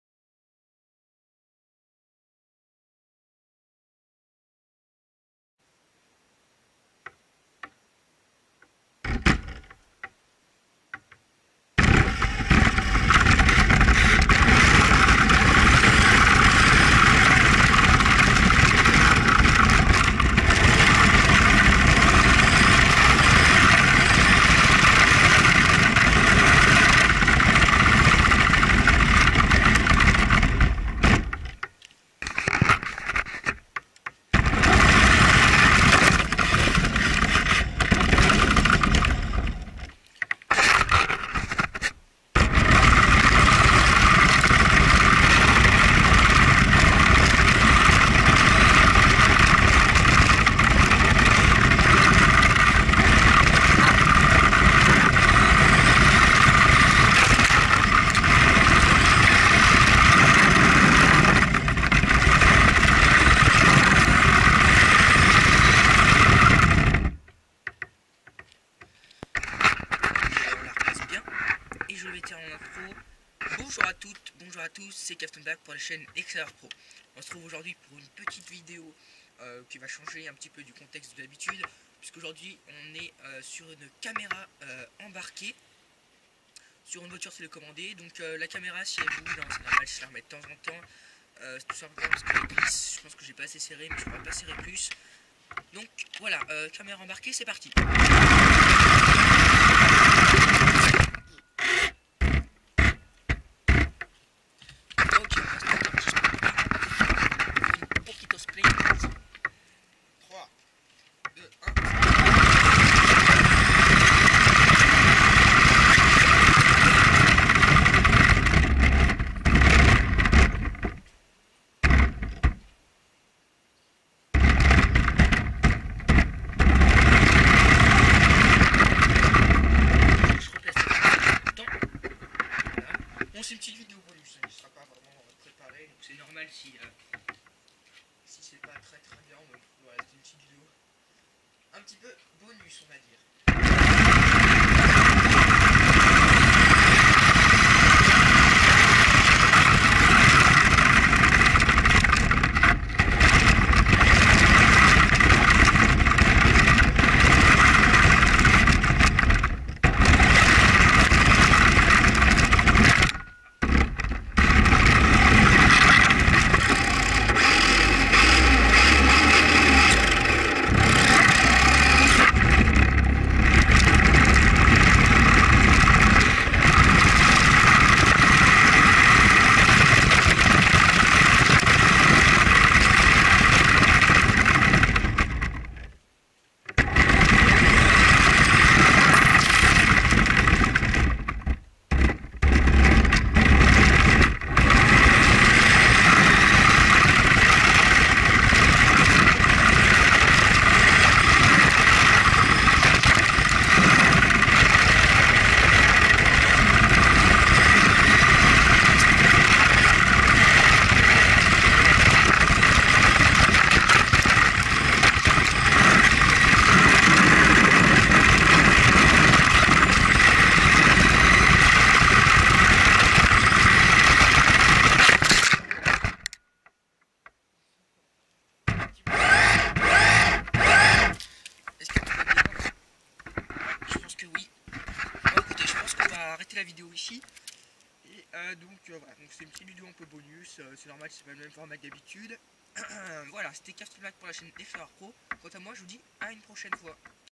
Camera embarquée sur une voiture télécommandée!
Une petite vidéo qui change de d'habitude. Je penses en faire plus souvent avec pourquoi pas des parcours organisés, des slaloms, des jump, des records de vitesse?